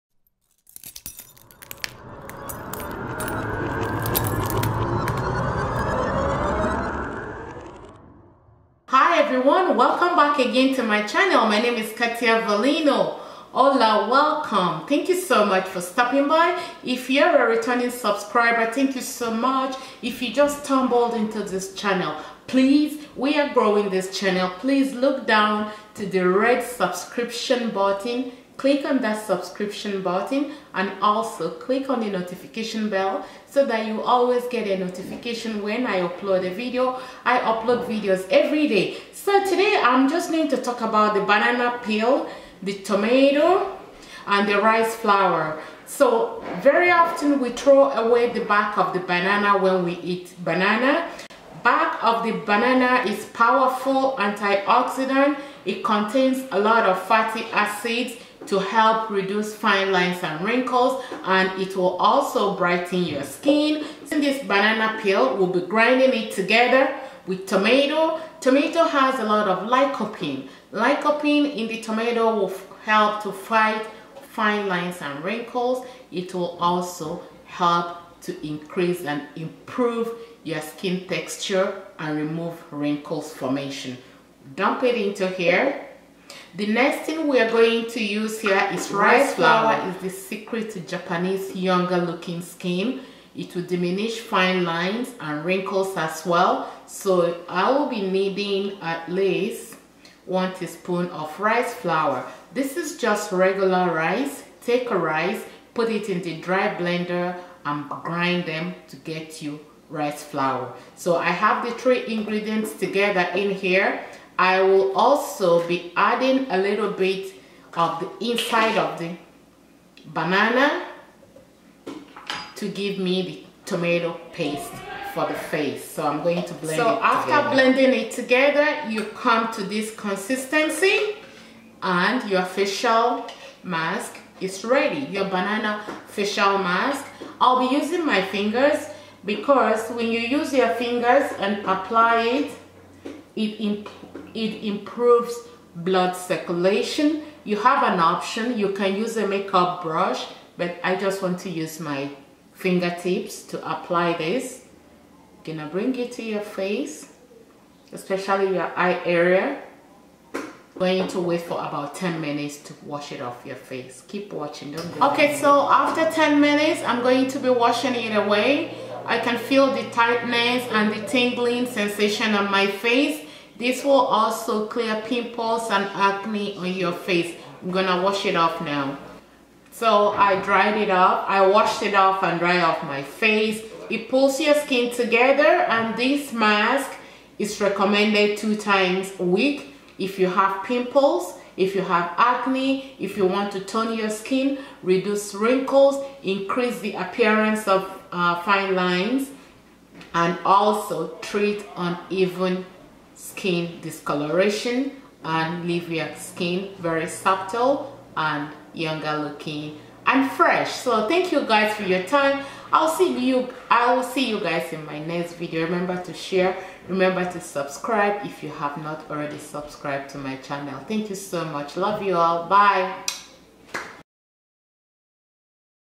STOP THROWING AWAY BANANA PEELS , MAKE FACIAL MASK WITH IT
In this video, you will learn how to use banana peels to make facial mask . Banana peels has many fatty acid and antioxidants that will help brighten your skin, even out skin discoloration , improve skin texture , l